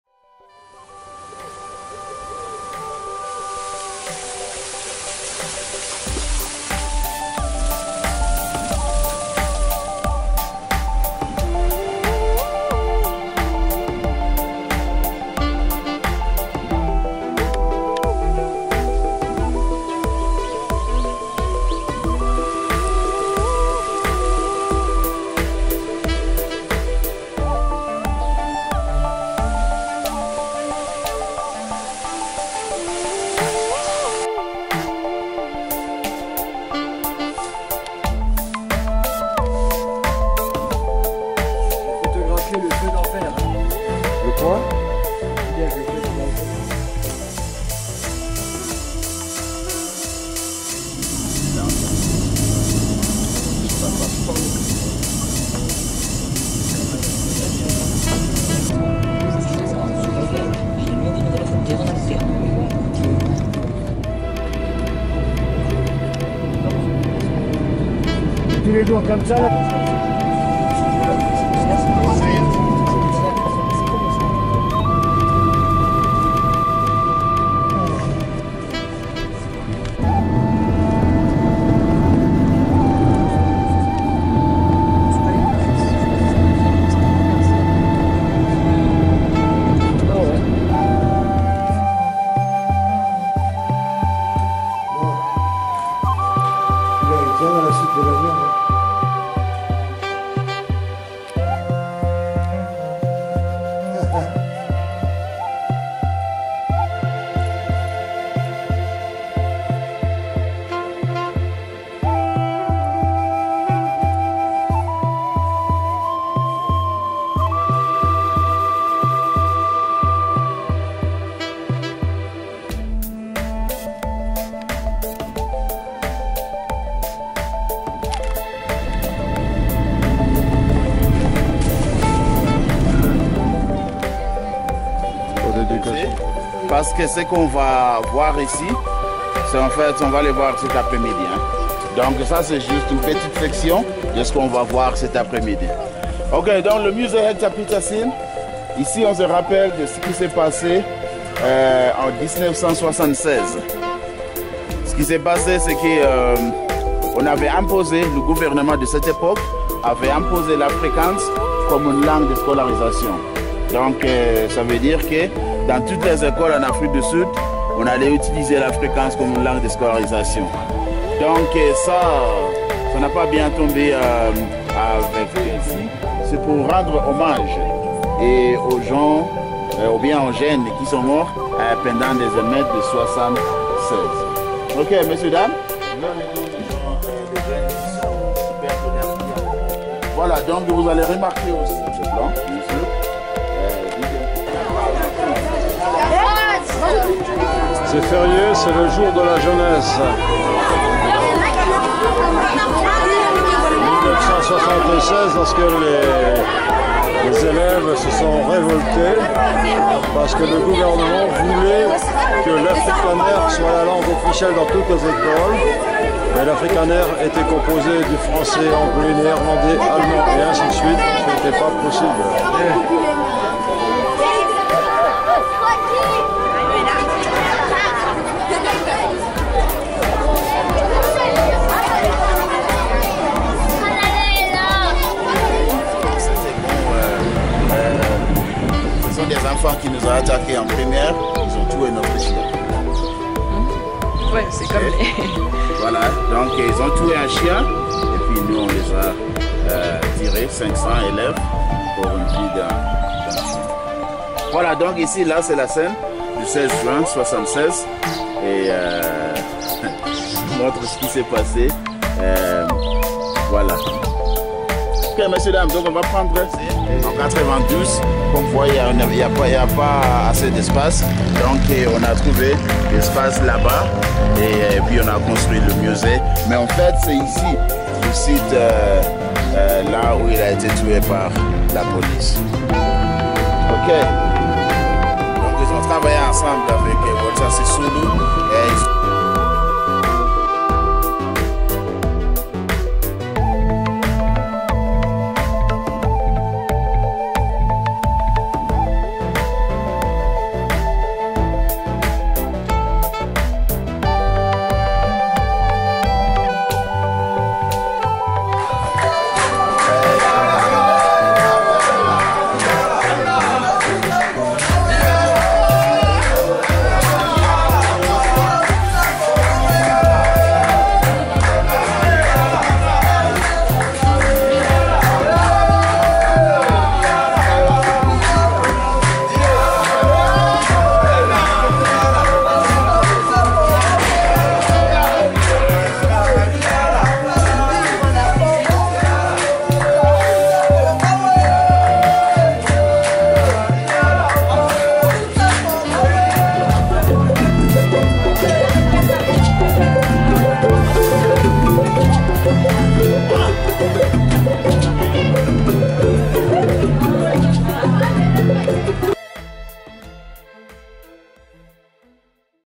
14 JOHANNESBURG – SOWETO – JOHANNESBURG
Découverte de Soweto. Visite du mémorial Hector Pietersen. Visite du Musée de l’Apartheid.